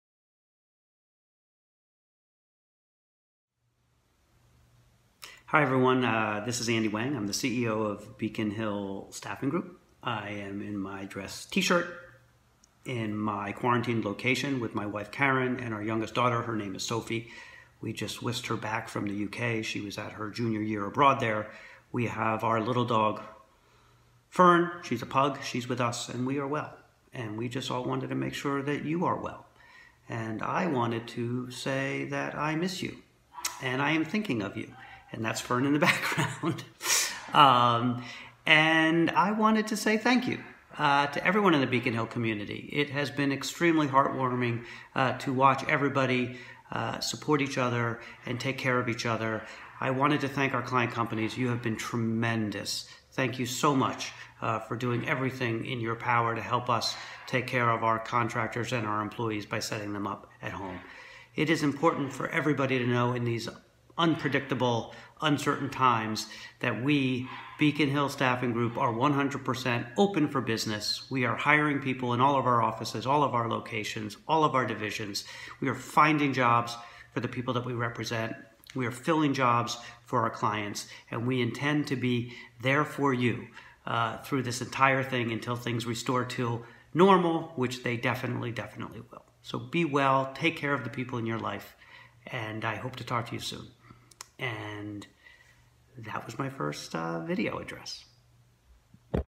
CEO Andy Wang discusses the Coronavirus Pandemic
Beacon Hill CEO Andy Wang recorded his first-ever video message to talk a bit about this tough time we’re all going through.